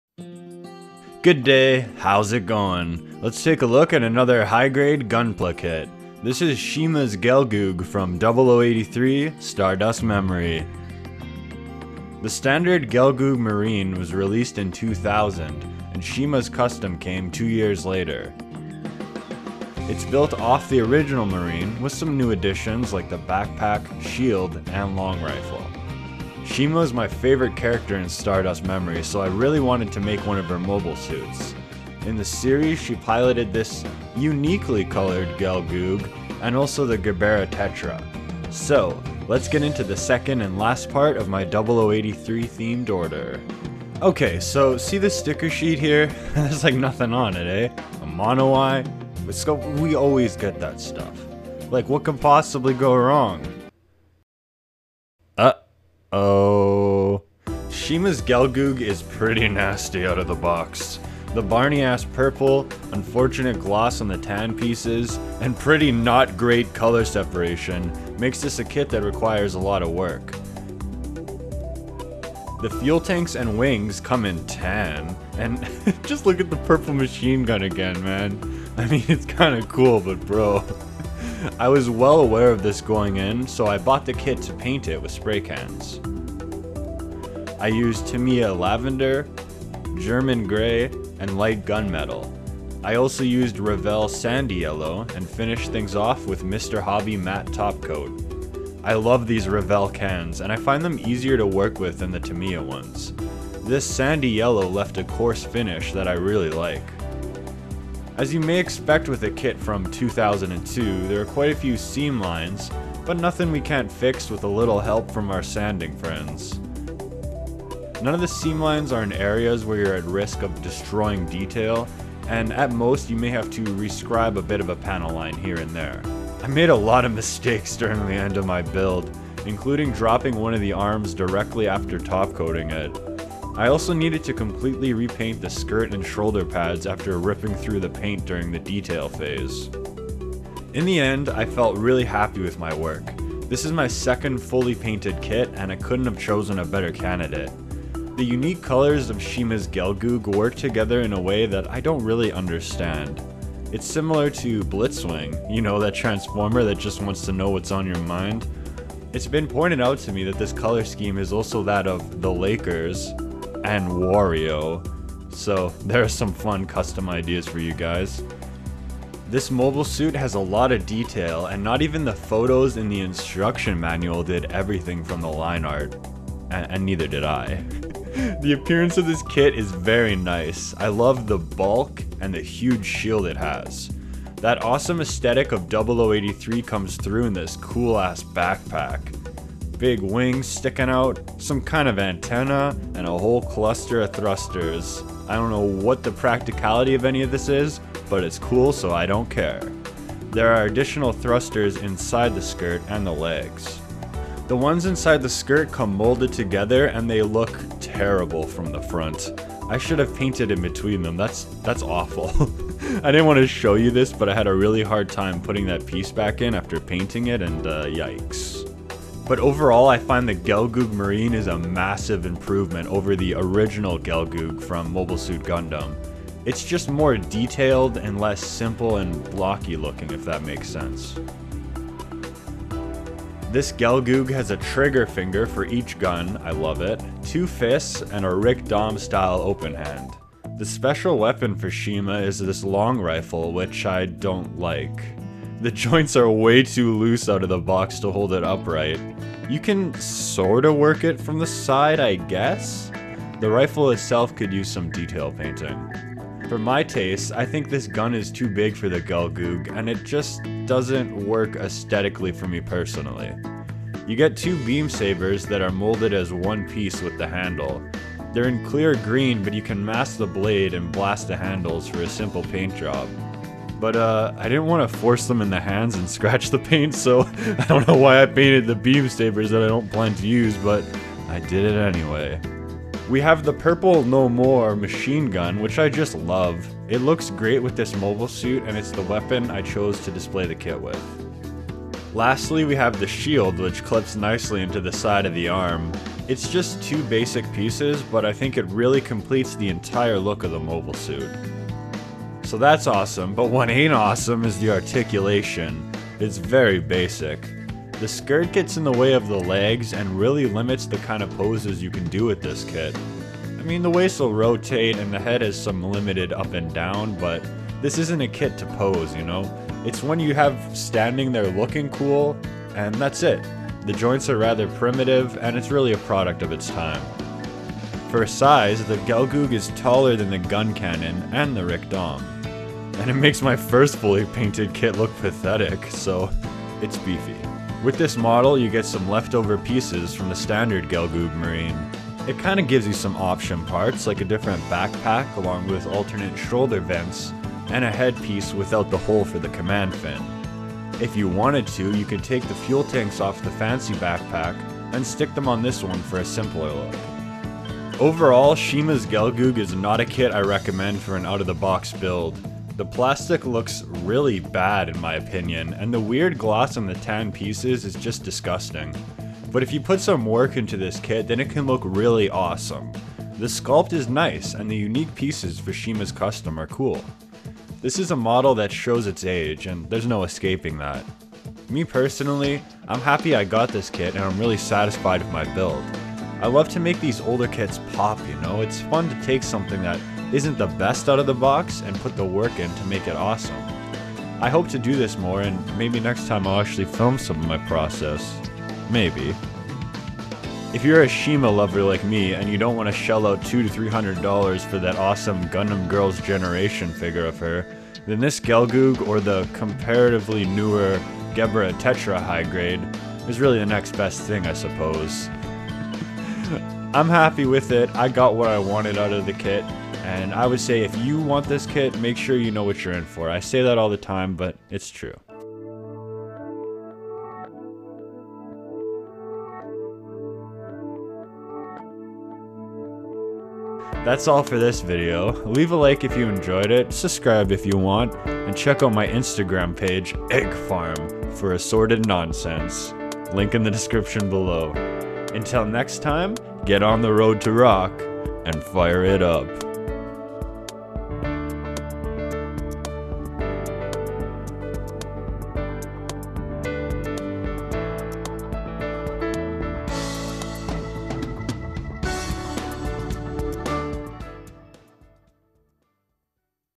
HGUC Gelgoog Marine Cima Custom | Model Kit Review
Let's look at Cima Garahau's unique Gelgoog Marine in this video.

Credit to Dalong for some of the photos used in this review